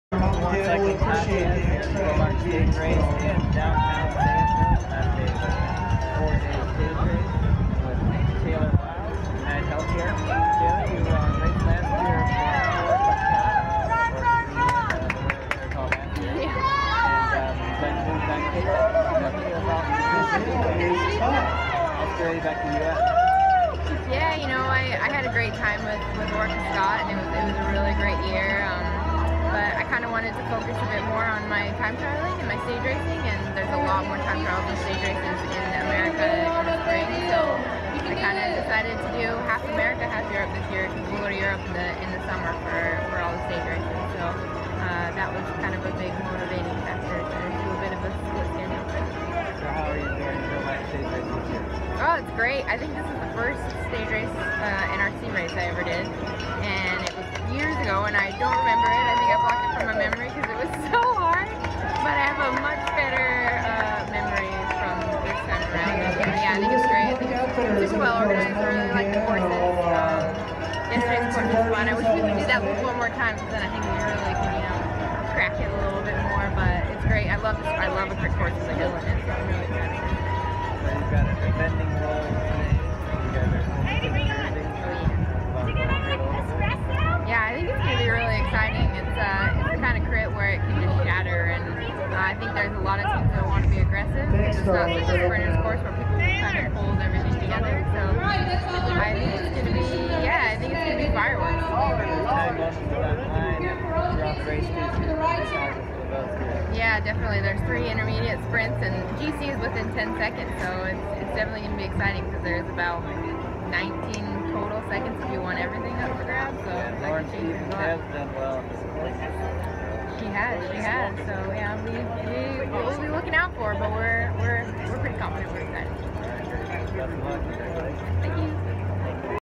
Tayler Wiles Joe Martin Stage Race Interview
UnitedHealthcare Pro Cycling's Tayler Wiles talks to the Warren Cycling Podcast before the final stage of the Joe Martin Stage Race.